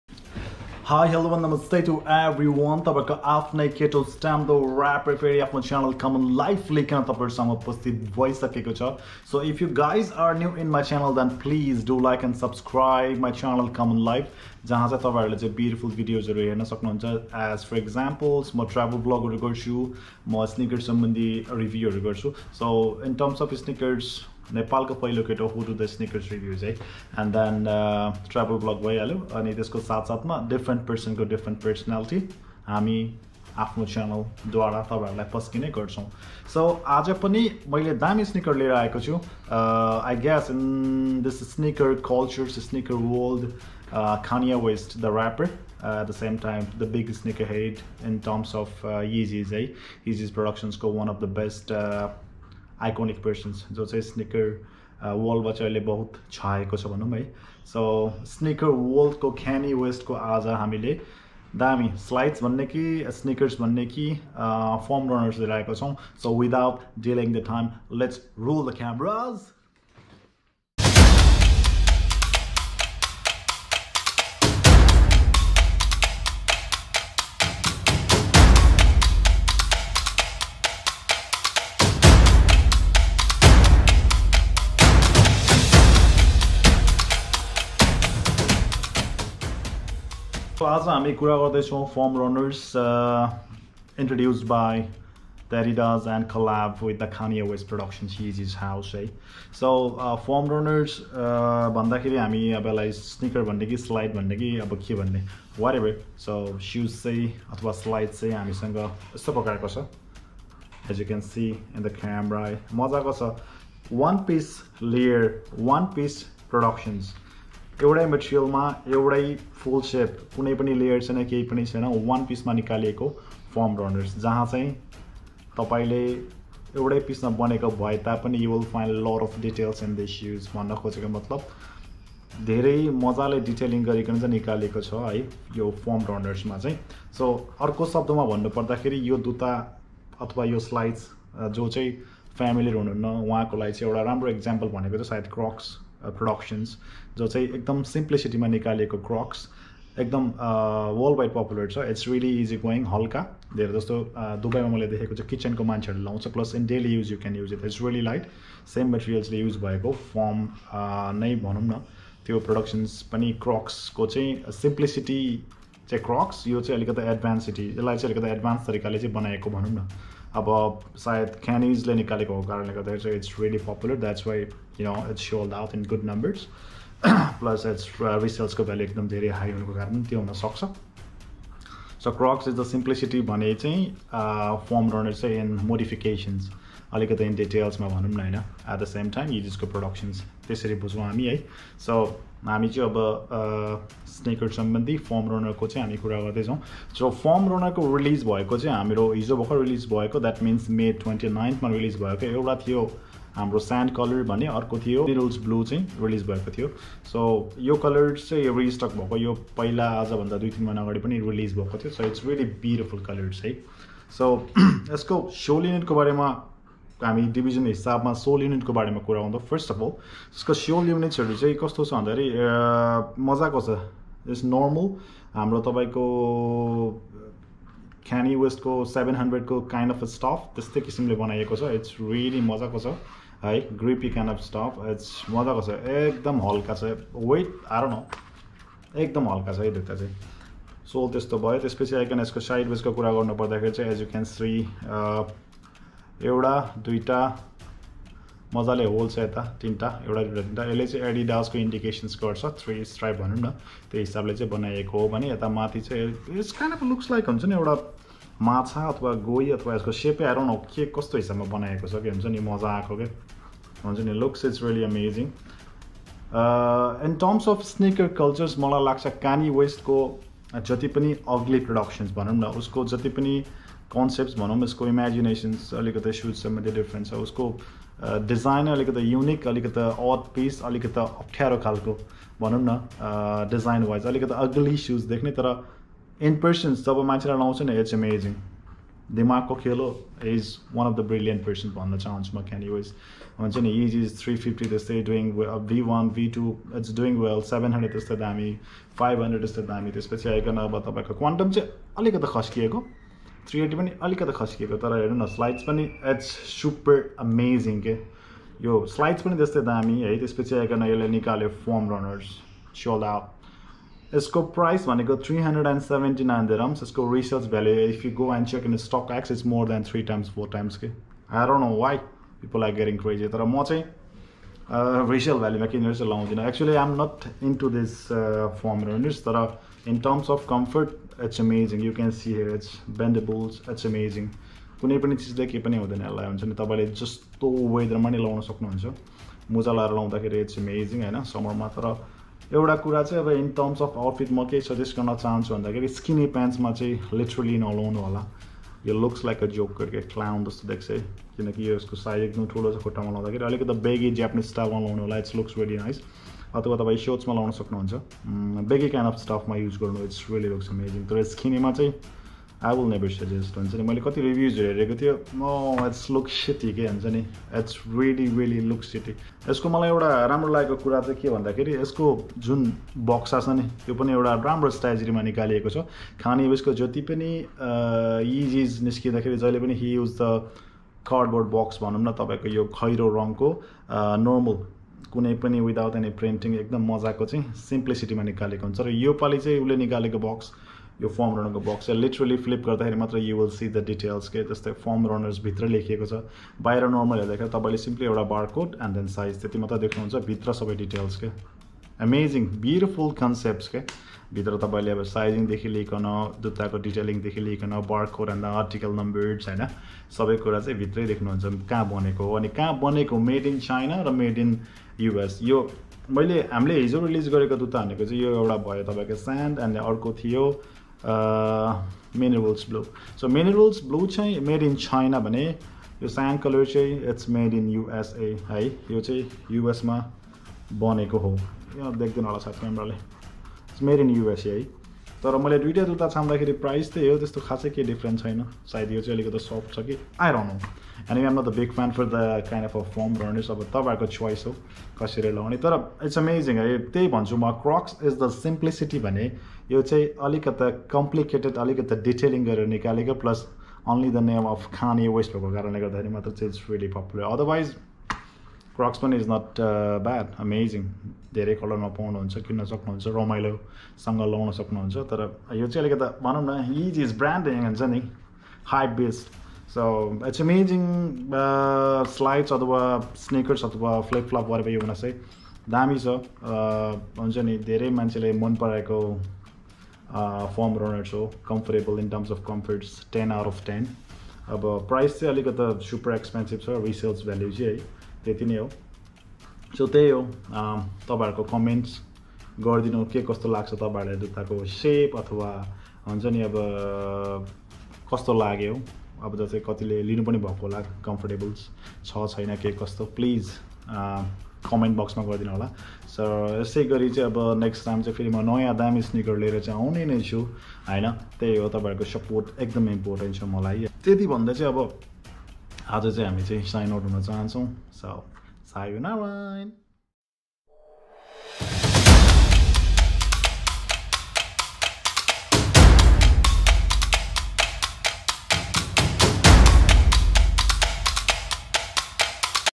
FOAM RUNNER SAND REVIEW
FORM RUNNER SAND REVIEW IS ONE OS MY BEAUTIFUL EXPERIANCE TO EXPLAIN THE BEAUTY N DETAIL IN MY OWN LANGUAGE NEPALI...LOVE TO BE THE SNEAKERHEAD REPRESENTING NEPAL..LOVE U ALL ...DO LIKE N SUBSCRIBE COMMON LIFE ALWAYS,,ADIDAS FORM RUNNER